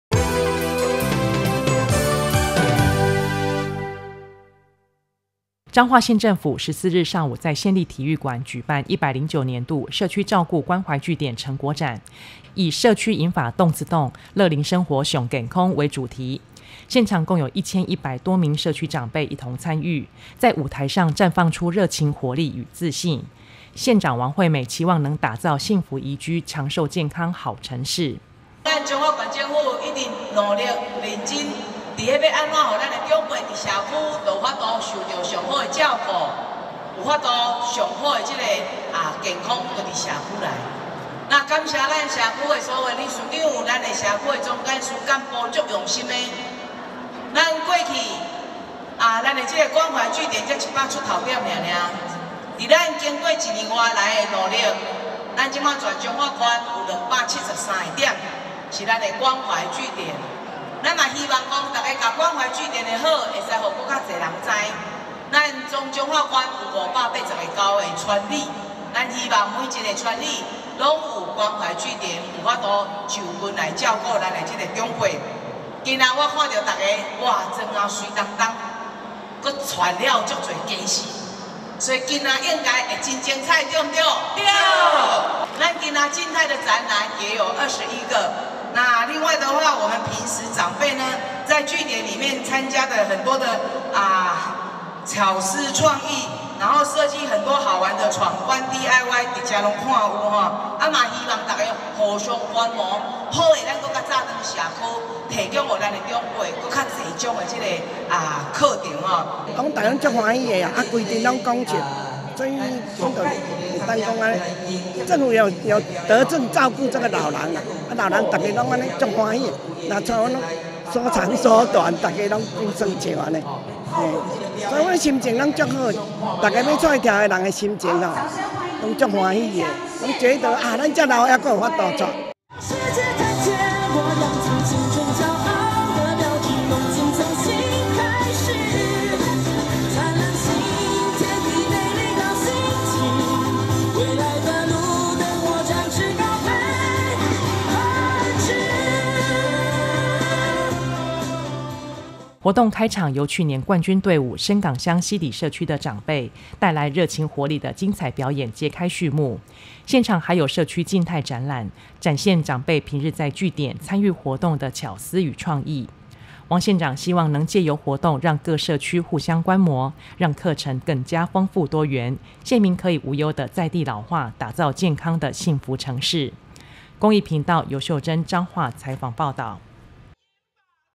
109-11-14 彰化社區關懷據點成果展 社區銀髮動次動 樂齡生活尚健康
彰化縣政府14日上午在縣立體育館舉辦「109年度社區照顧關懷據點成果展」，以「社區銀髮動次動 樂齡生活尚健康」為主軸，現場共有1,100多名社區長輩一同參與，在舞台上綻放出熱情活力與自信，縣長王惠美期望能打造幸福宜居、長壽健康好城市。
活動開場由去年冠軍隊伍伸港鄉溪底社區的長輩，帶來熱情活力的精采表演揭開序幕，現場還有社區靜態展覽，展現長輩平日在據點參與活動的巧思與創意，王縣長希望能藉由活動讓各社區互相觀摩，讓課程更加豐富多元，縣民可以無憂的在地老化，打造健康的幸福城市。